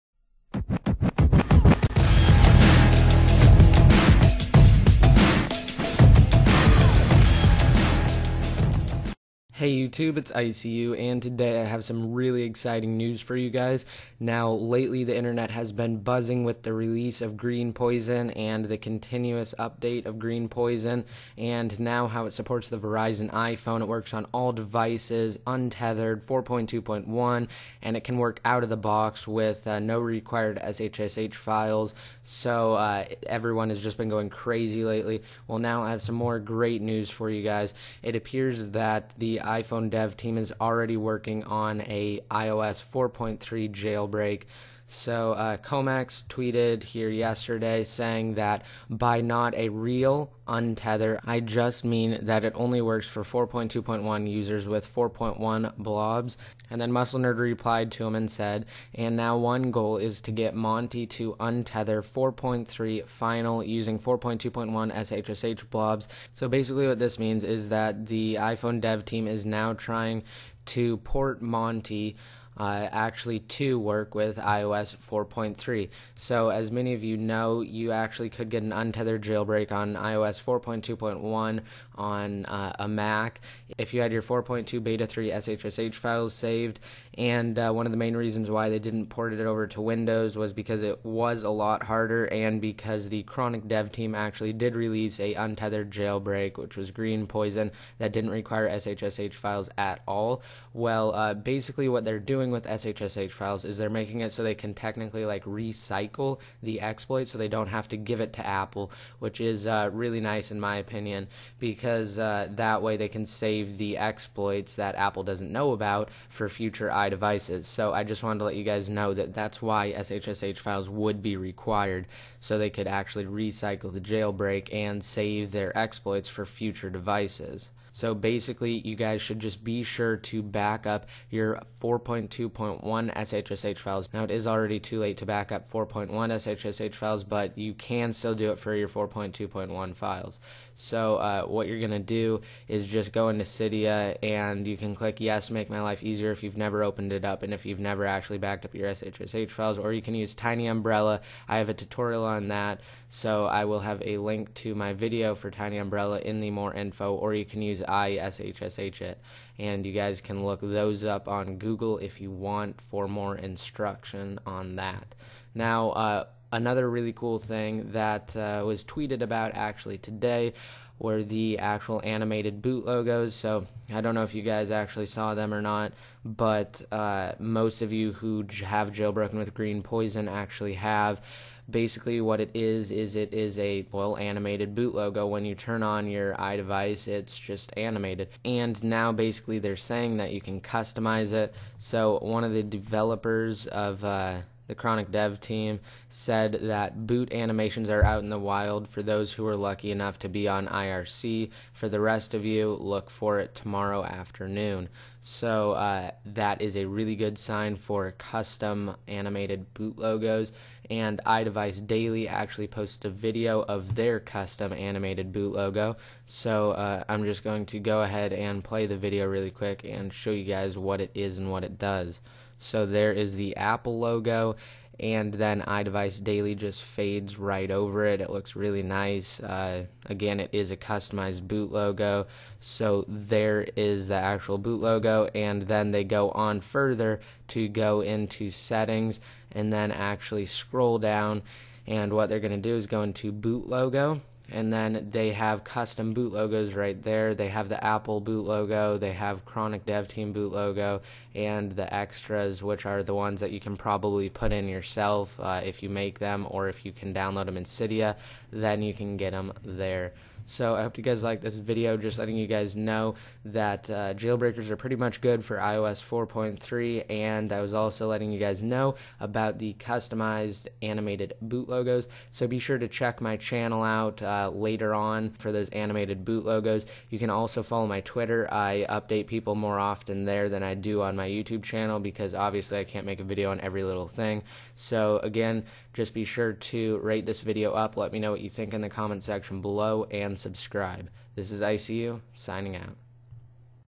iOS 4.3 Will Be Jailbroken AND NEW Customized Animated Boot Logos
Yeh thats right you heard it, iOS 4.3 WILL BE JAILBROKEN (most likely SOON after it's public debut).

Make sure you back up your 4.2.1 SHSH files here is a video that will tell you how to do it